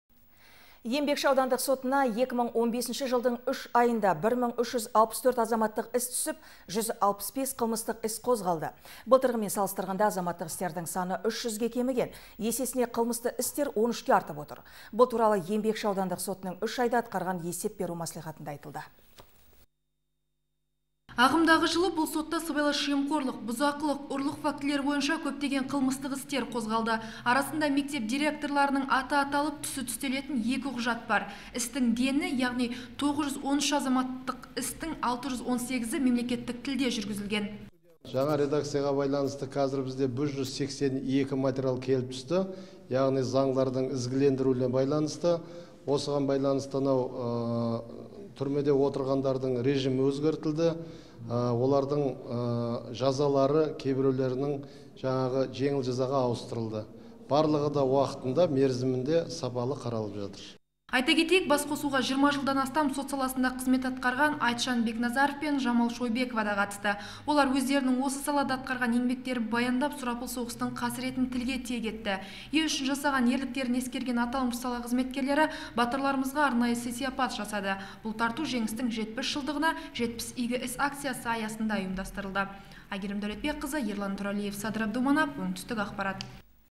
Былтырғымен салыстырғанда азаматтық істердің саны 300-ге кеміген
Еңбекші аудандық сотына 2015 жылдың 3 айында 1364 азаматтық іс түсіп, 165 қылмыстық іс қозғалды. Былтырғымен салыстырғанда азаматтық істердің саны 300-ге кеміген. Есесіне, қылмыстық істер 13-ке артып отыр. Бұл туралы  Еңбекші аудандық сотының 3 айда атқарған есеп беру мәслихатында айтылды.